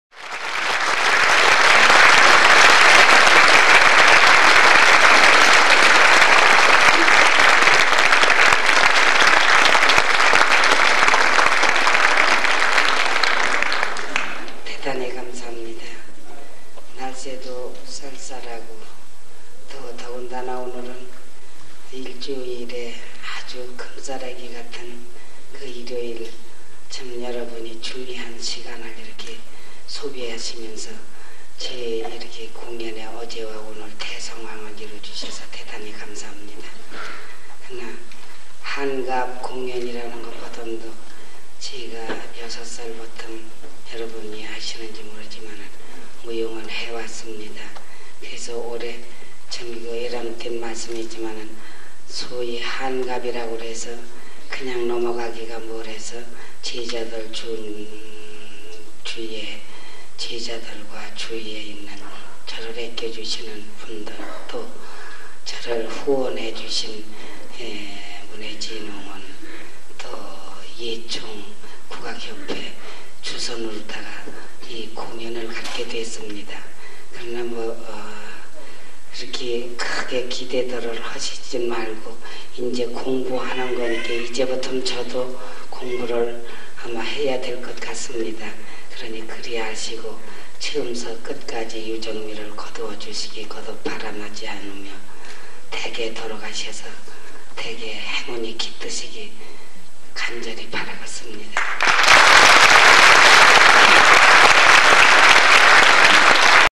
김숙자 선생 인사말
행사:김숙자 회갑기념공연
일시:19861102
장소:문예회관 대극장
출연:김숙자